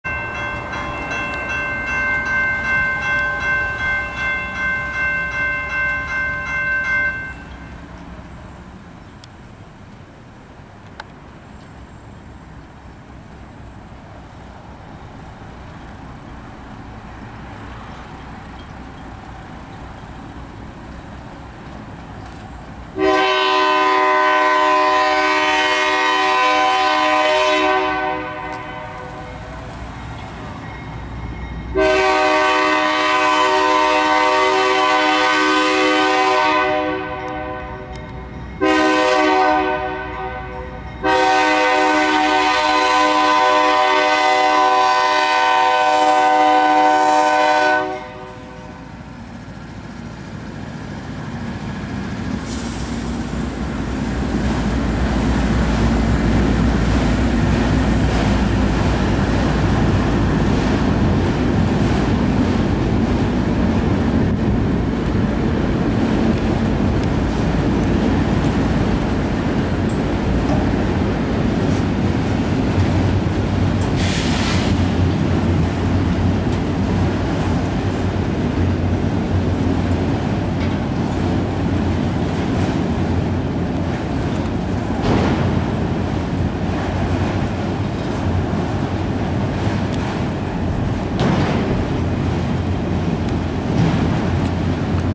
03/22/24 - BNSF warbonnet 769 leads NS 16E solo in Centralia, Illinois
After a long night at work I got heads up that NS train 16E with autoracks was being lead solo by BNSF warbonnet 769. This felt like some early 2000s vibes and the Mexican tuned K3LA on the unit made this catch even better. Enjoy!